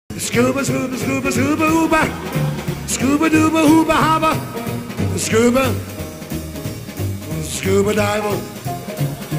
SCUBA FOR $35 US! Are you for scuba?
We found a place in Cuba where you can scubadive for $35 US all in. This was my first time ever scubadiving. Cheers!